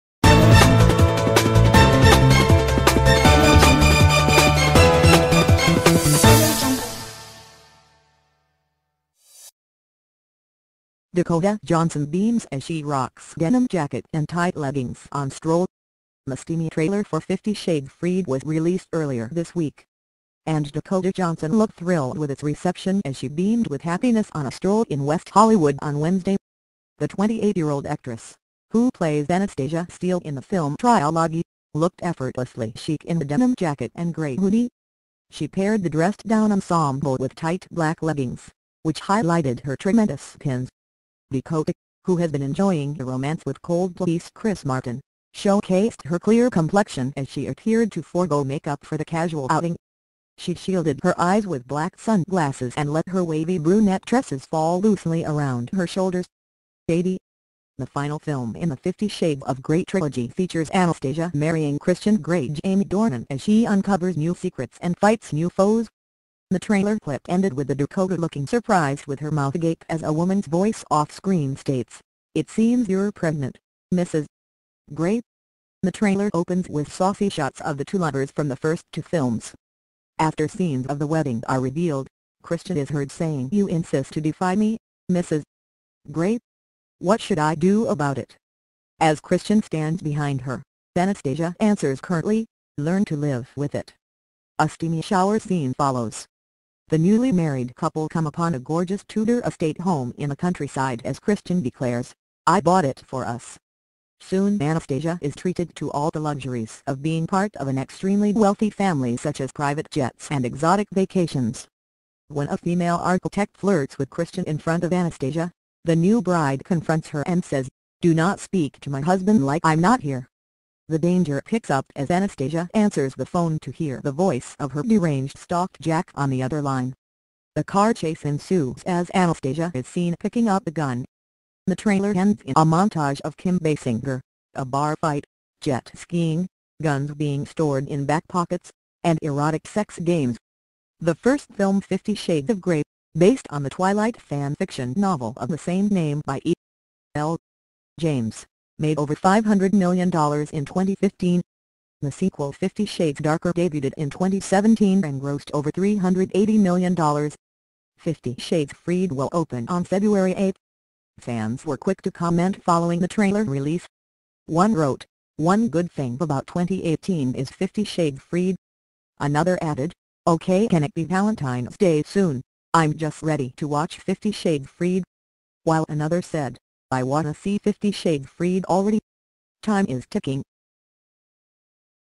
Dakota Johnson as she rocks denim jacket and tight leggings on stroll
The trailer for Fifty Shades Freed was released earlier this week.
And Dakota Johnson looked thrilled with its reception as she beamed with happiness on a stroll in West Hollywood on Wednesday.